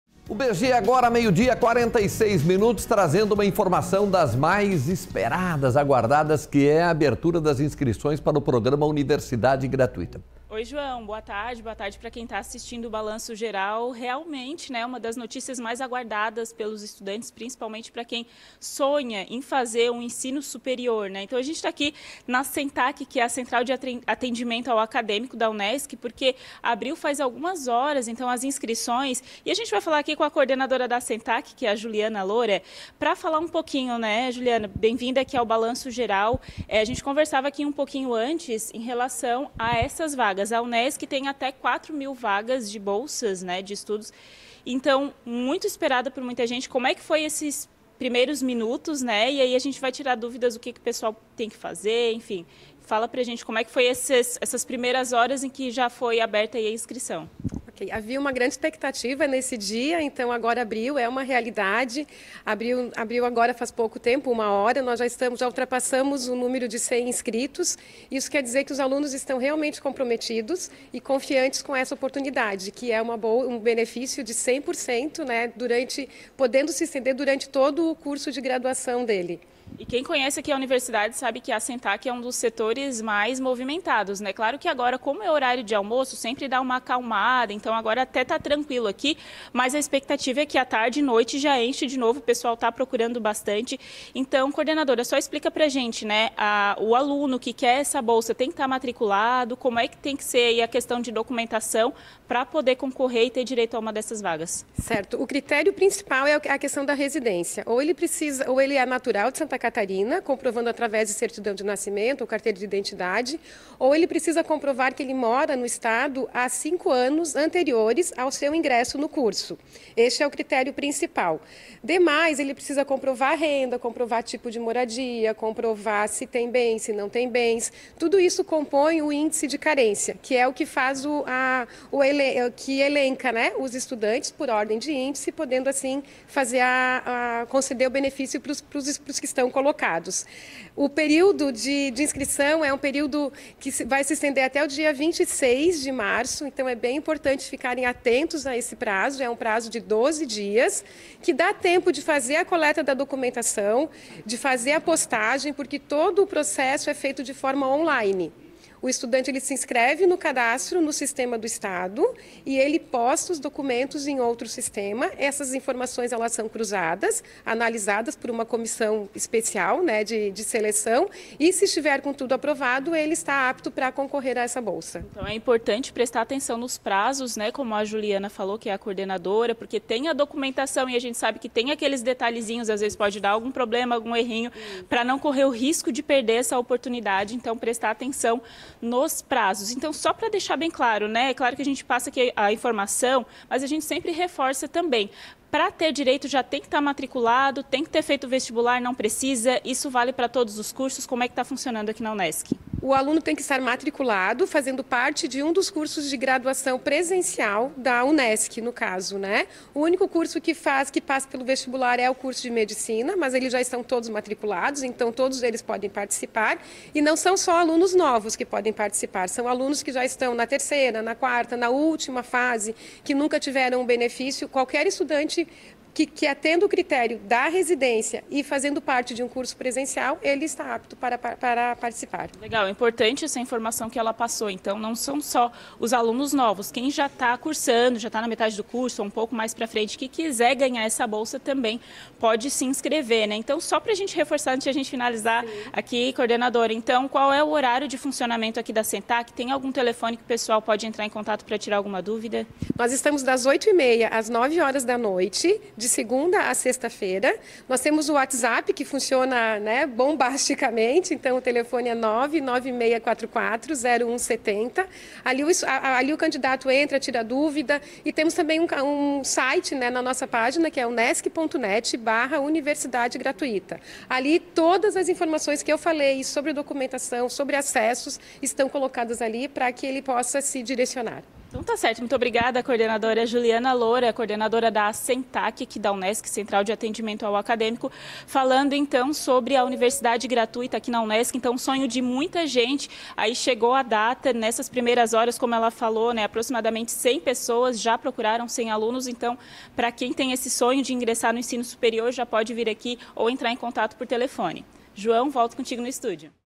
Universidade Gratuita: inscrições abertas para oportunidades de ensino
ão perca tempo! As inscrições para o Programa Universidade Gratuita estão abertas a partir de hoje. Emanuela Justino traz todas as informações em tempo real direto da UNESC. Saiba como aproveitar essa oportunidade de acesso à educação de qualidade sem custos.